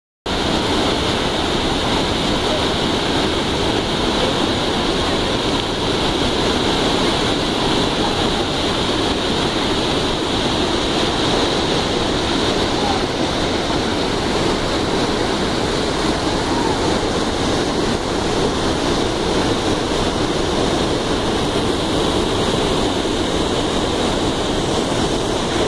Piano Key Weir at Giritale Reservoir, Sri Lanka
First PKW Spillway constructed in Sri Lanka spilling on 21st December 2014.